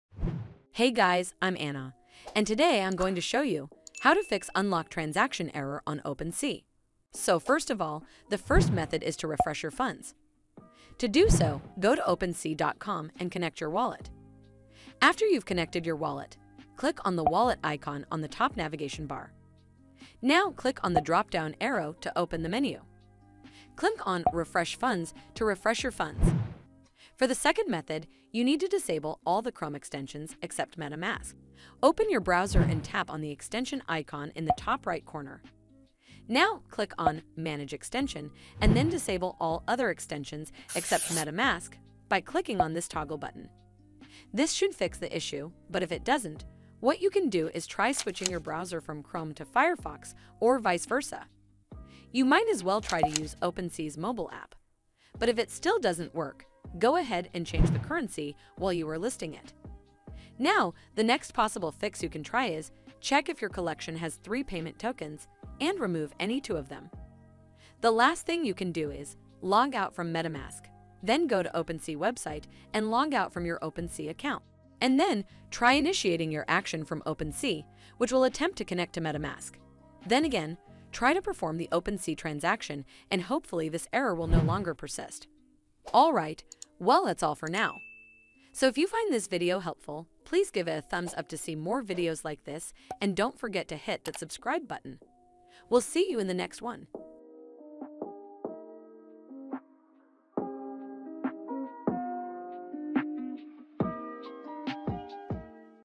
How to Fix Unlock Currency Transaction Error on OpenSea (FIXED) | 2022 | Bytes Media
Are you unable to unlock currency on OpenSea? Here’s the error message, “There was an error with your transaction. Please try again“. Some people managed to fix the error by trying to unlock the currency multiple times. However, others failed to unlock the currency no matter how many times they’ve tried. Before you attempt to fix the error, you need to check if OpenSea is down. In this video, I'll show you How to Fix Unlock Currency Transaction Error on OpenSea. This is the easiest and fastest way to Fix Unlock Currency Transaction Error on OpenSea. Make sure you watch until the end of this video to find out How to Fix Unlock Currency Transaction Error on OpenSea. Hope you enjoy!